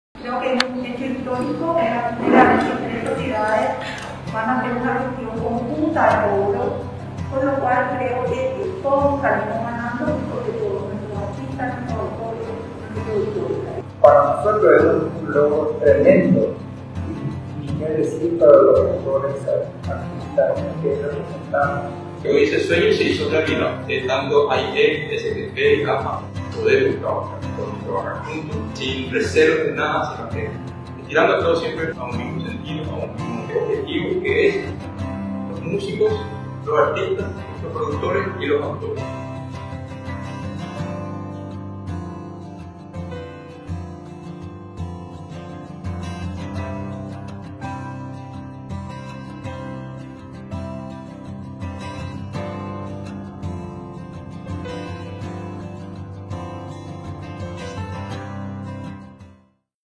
Convenio APA, SGP y AIE
Las tres sociedades de gestión APA AUTORES PARAGUAYOS ASOCIADOS, SGP Paraguay y AIE Paraguay se unieron firmando el convenio para percepción conjunta, logrando mayor eficacia en el cobro, para la posterior distribución de los derechos de autor y derechos conexos.